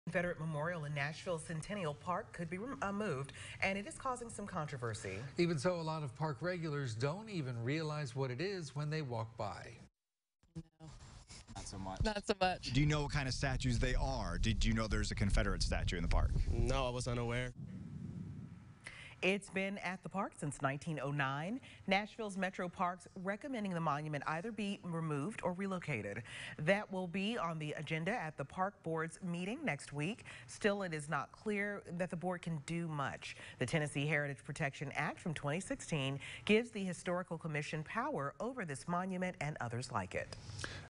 Nashville officials to discuss removing confederate monument from city park
Nashville Metropolitan Parks and Recreation will discuss removing or relocating a confederate monument from Centennial Park.